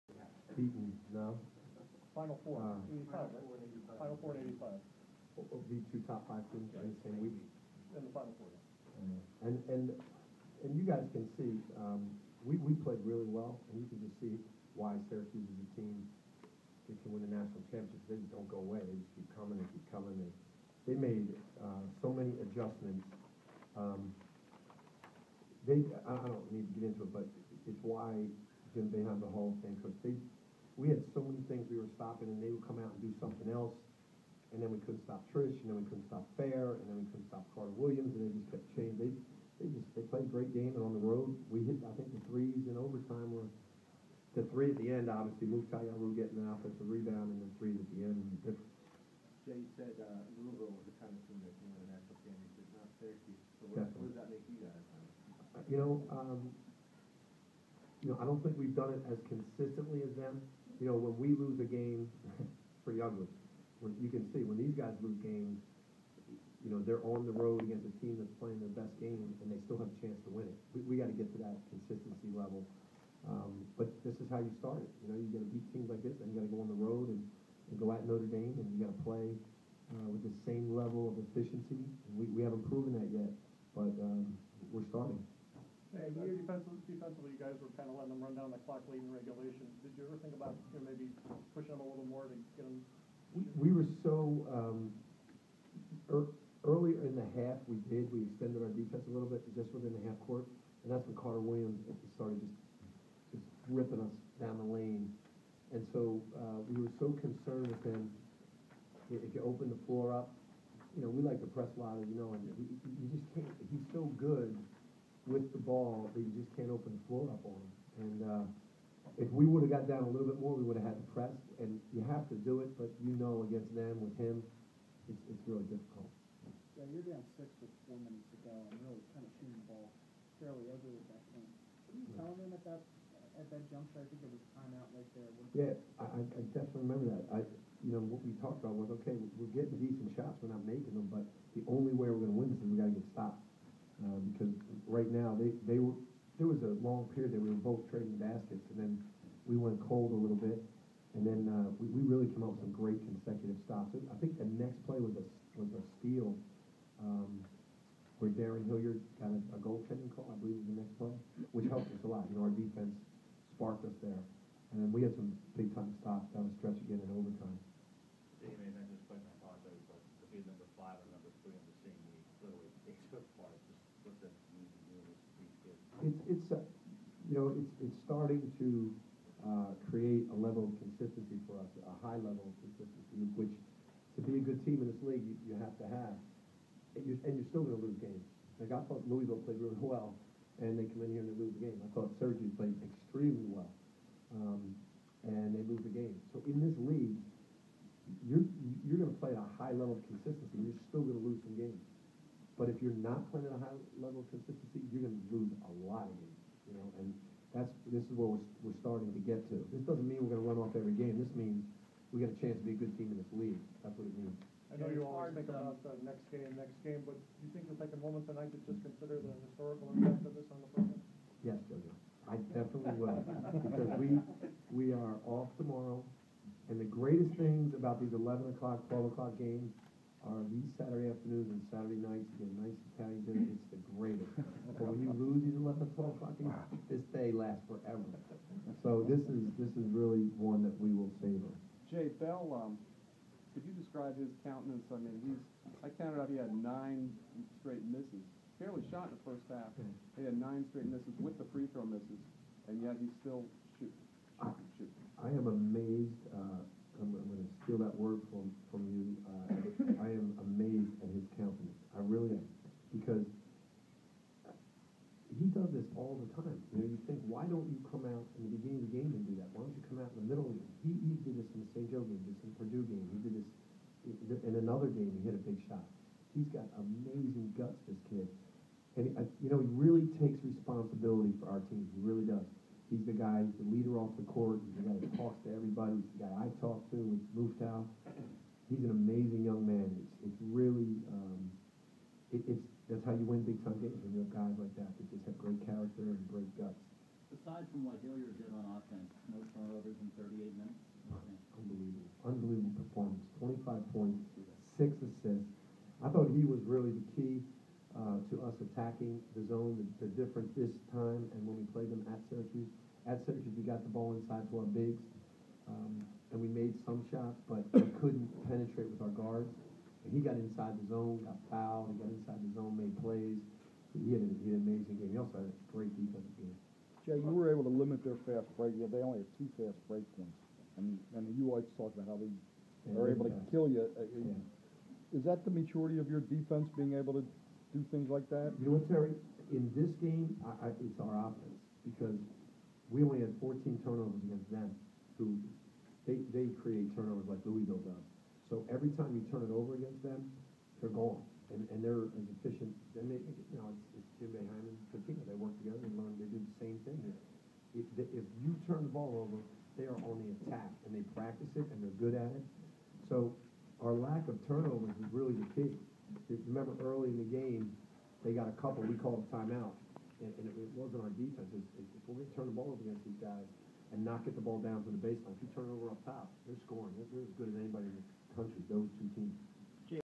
Jay Wright after Mac beats No. 3 Cuse
Wright addresses the media following Villanova's second consecutive win over a top 5 team in five days